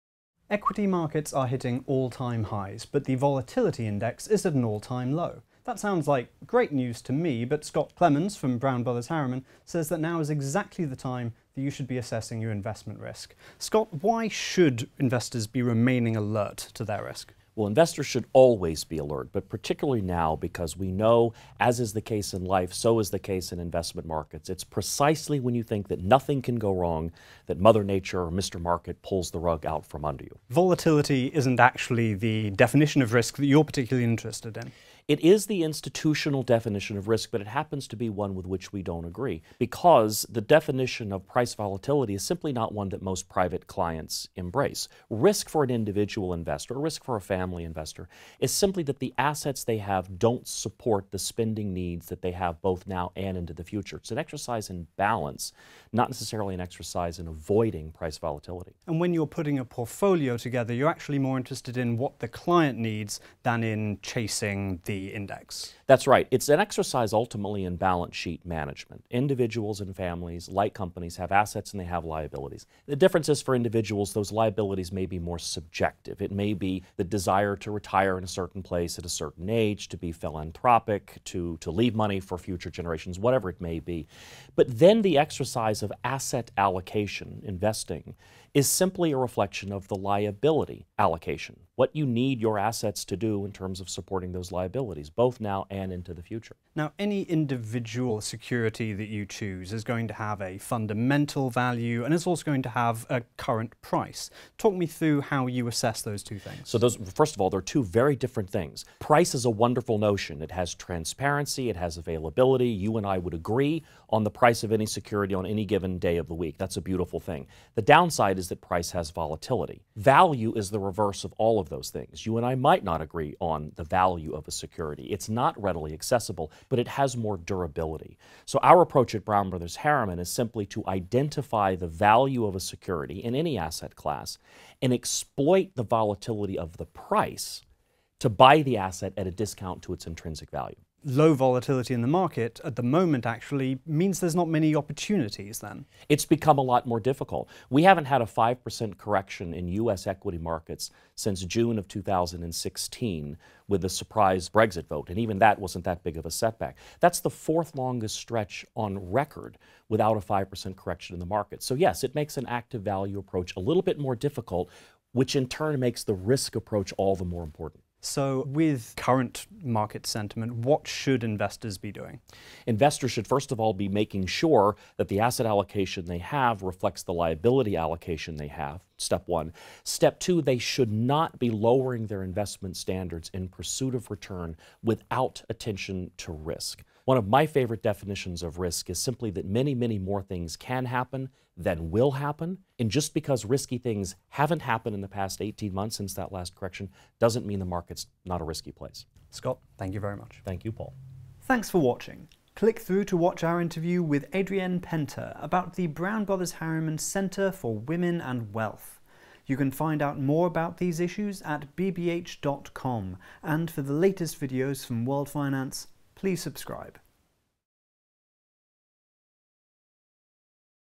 BBH: Manage risk to match your needs, not to avoid price volatility | World Finance
Equity markets are hitting all-time highs, while the volatility index is at an all-time low. Although this sounds like positive news, for Scott Clemons from Brown Brothers Harriman, now is exactly the time that you should be assessing your investment risk. He explains why BBH rejects the institutional definition of risk, the private bank's approach to portfolio selection, and why low volatility is making his active value approach harder.